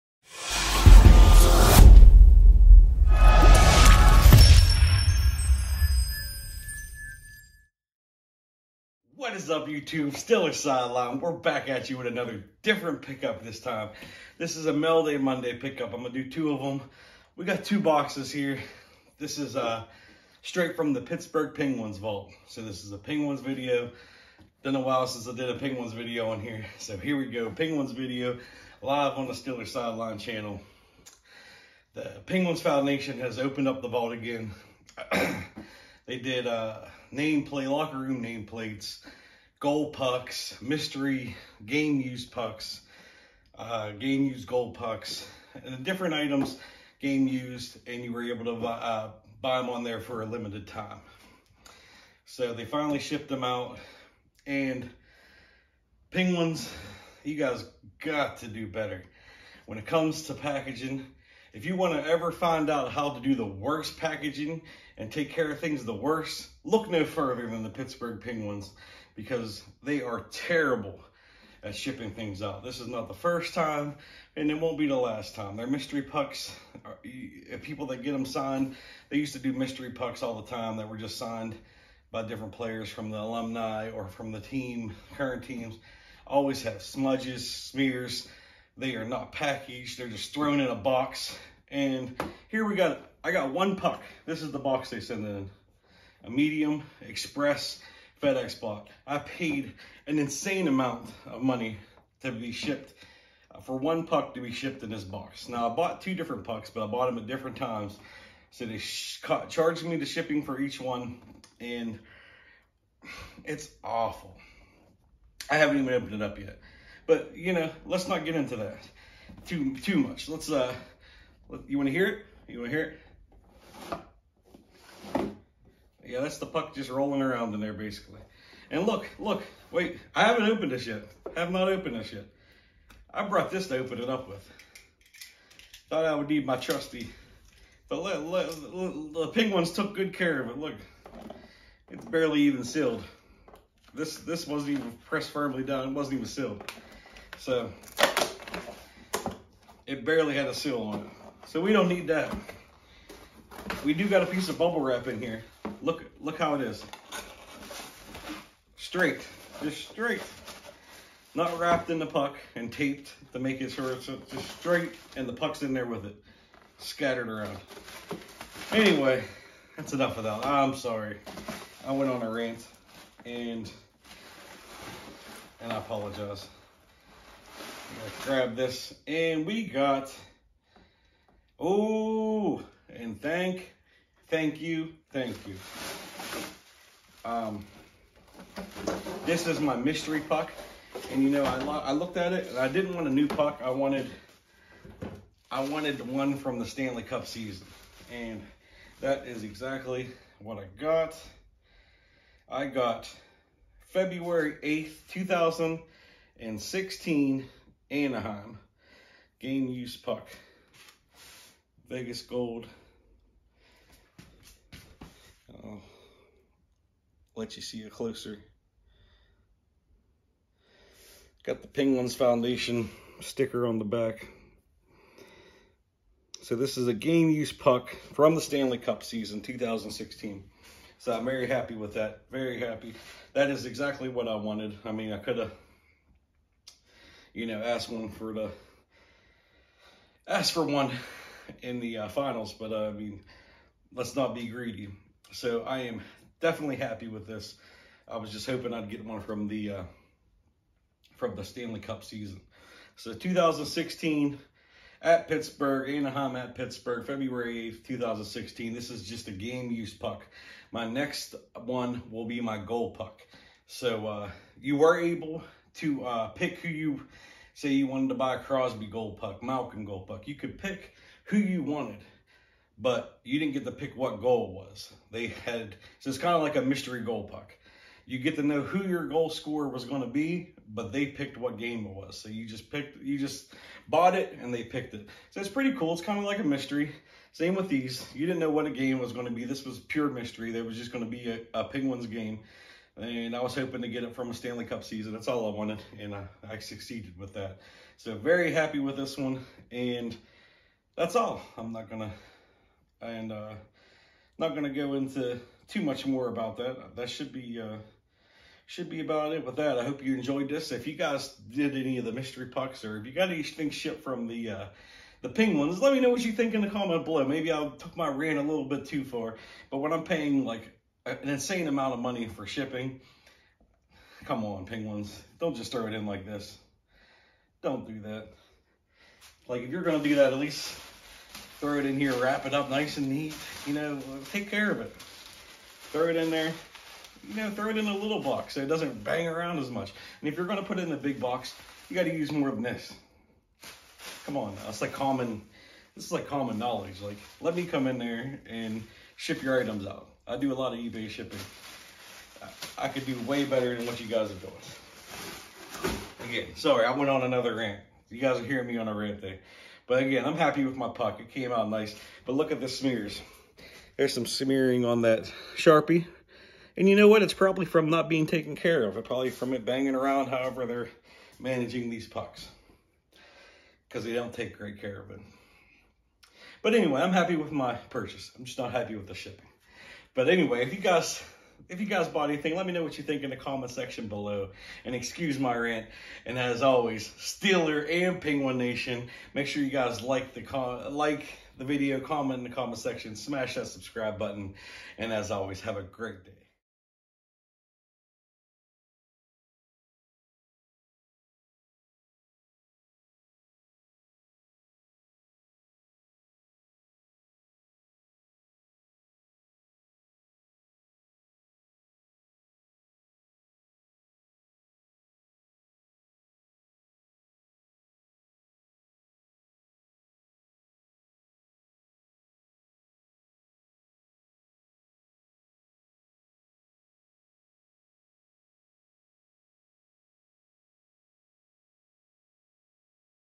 Penguins Vault Game Used Mystery Puck Unboxing
Hey guys! In this video I will be showing you my recent pickups! This one from penguins vault!  Check out the video for more!